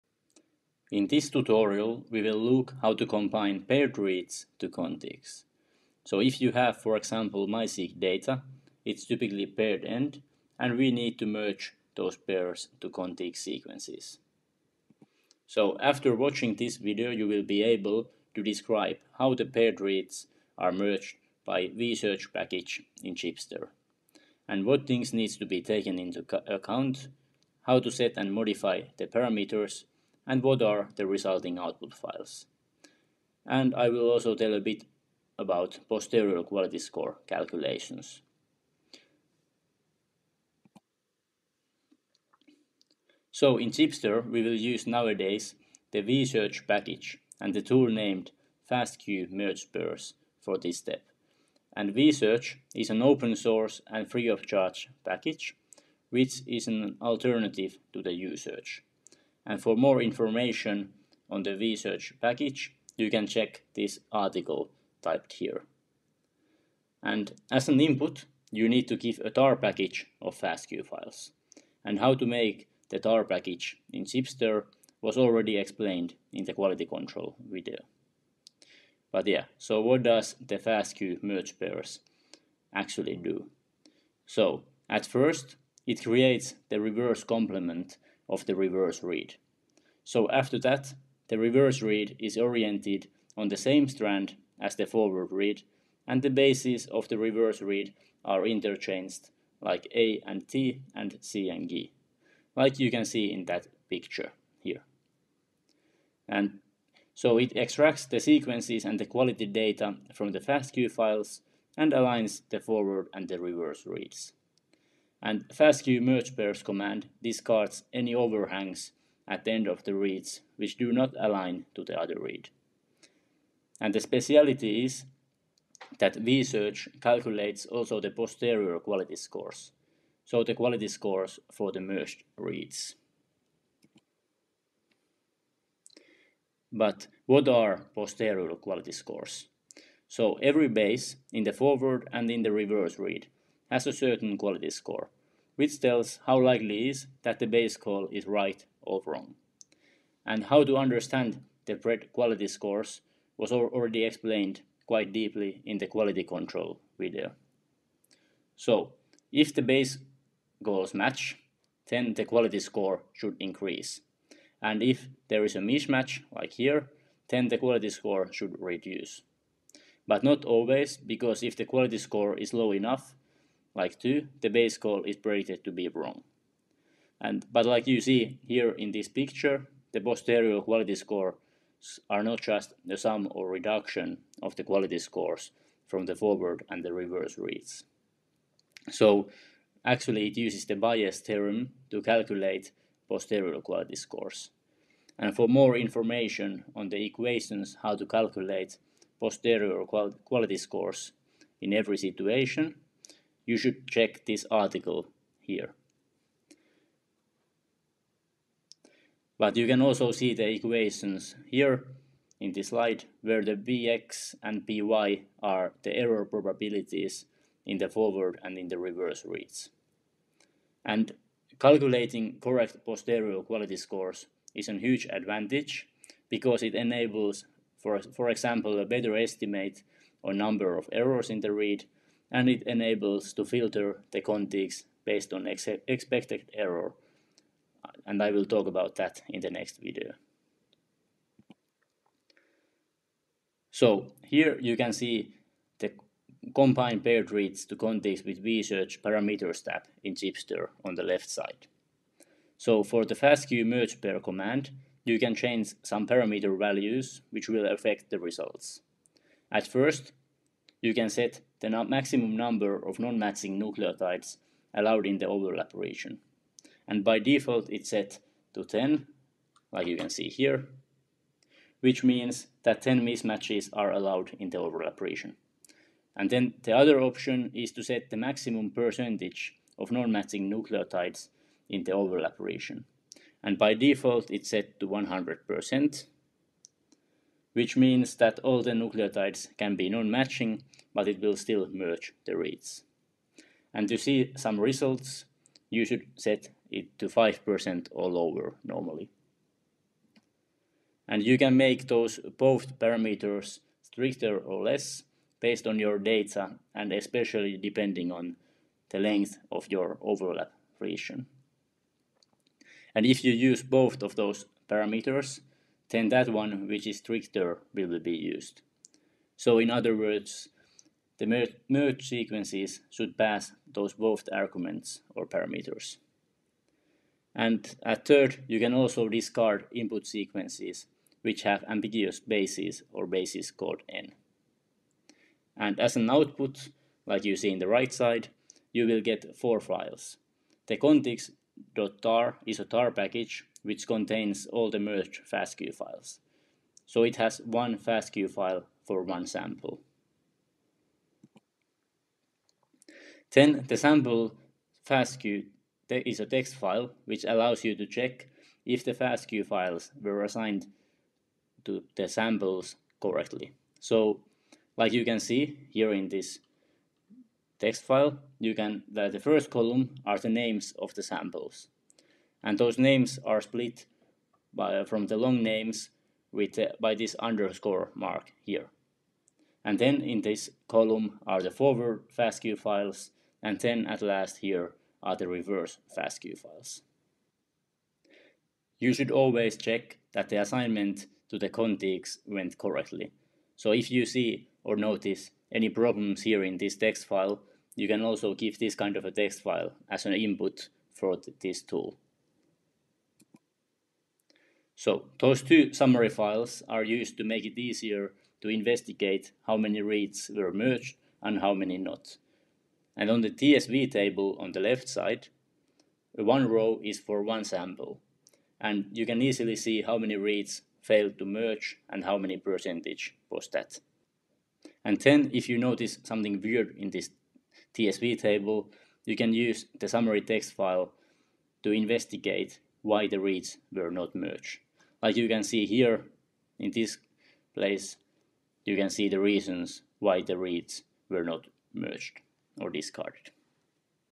3. Combine paired reads to contigs with VSEARCH
This video describes how are paired reads merged to contigs with VSEARCH in Chipster. You will learn about the merging process and posterior quality score calculations.